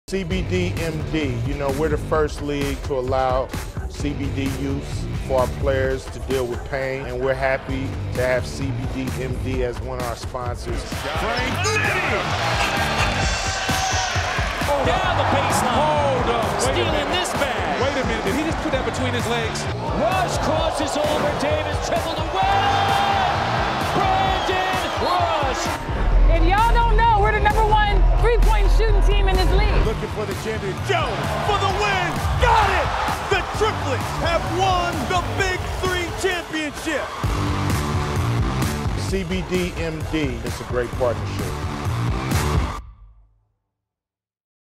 BIG3 + cbdMD: 2019 Season Recap
After an exciting first year, cbdMD & the BIG3 look back on the 2019 season.